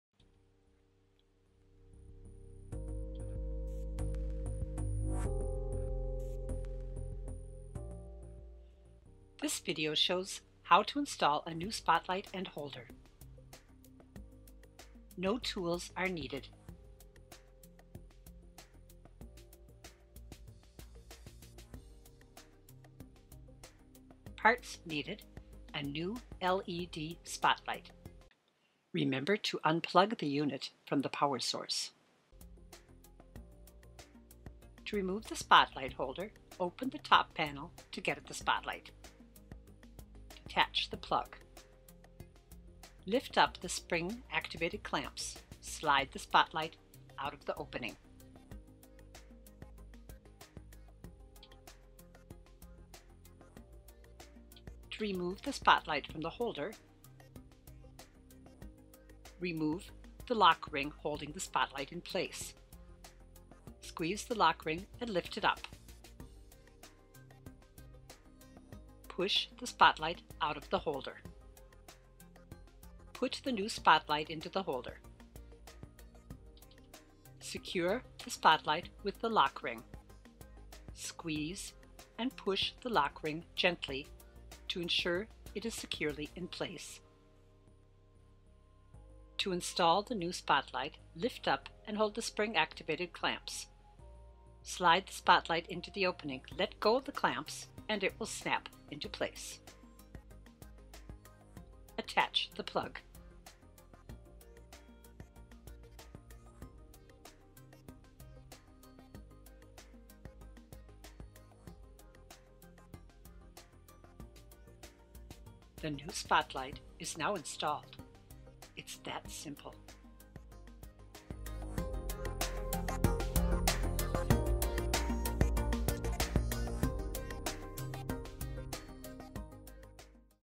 Replacing the spotlight (ULUF)
A guide on how to change the spotlight in an ULUF unit.